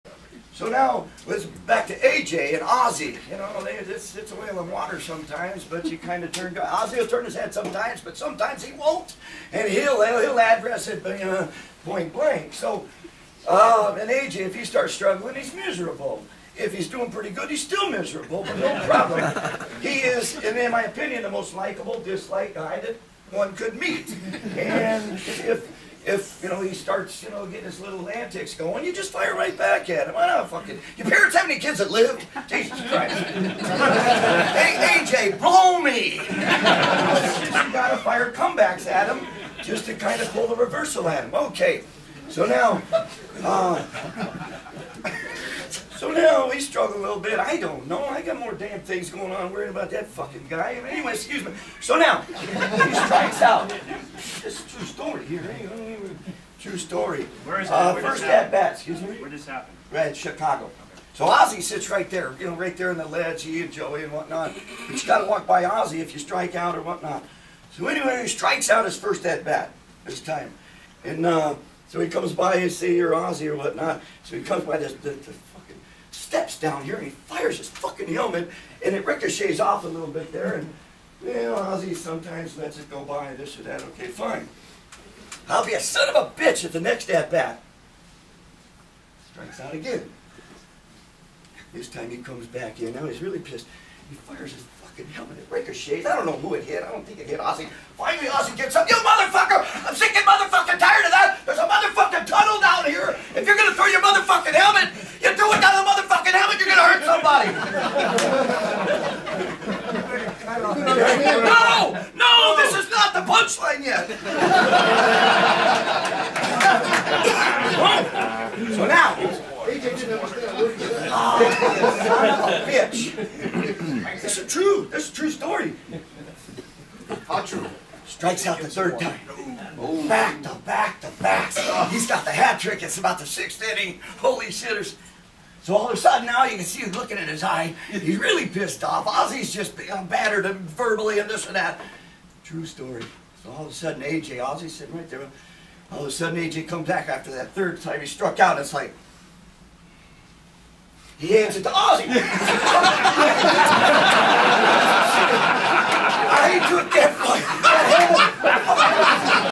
Ozzie Guillen and AJ Story
The newly named Marlin Manager Ozzie Guillen has a way of handling his players. Even difficult players like A.J. Pierzinski.  Thirdbase coach Jeff Cox of the Chicago White Sox tells of one such instance.